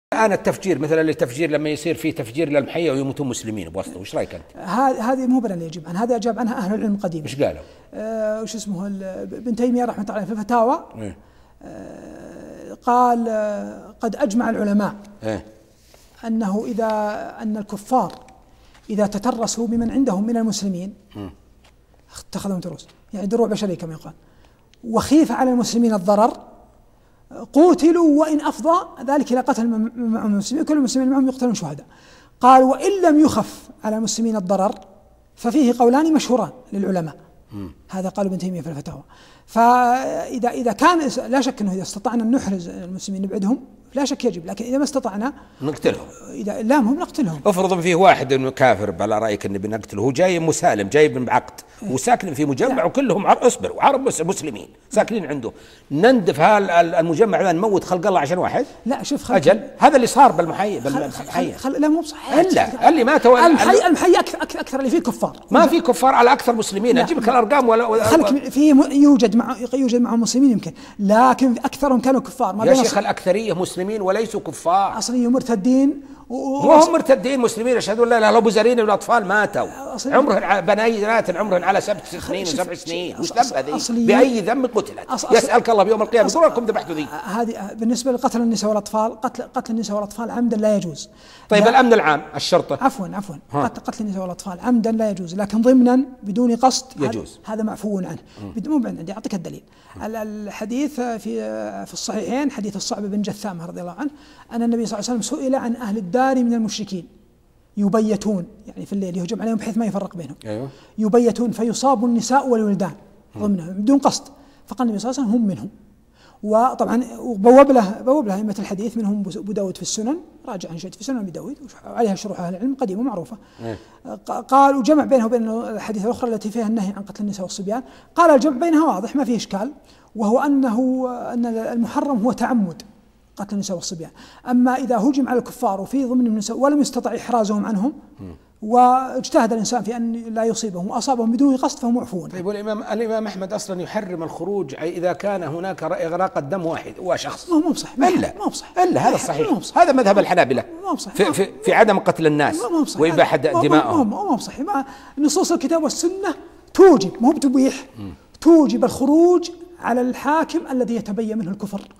سكان مجمع المحيا كفار .. وقتل النساء والأطفال معفو عنه
وليد السناني: سكان مجمع المحيا كفار .. وقتل النساء والأطفال معفو عنه
حلقة لقاء وليد السناني
الأحد 2013/11/17م

منتج الحلقة: نواف العضياني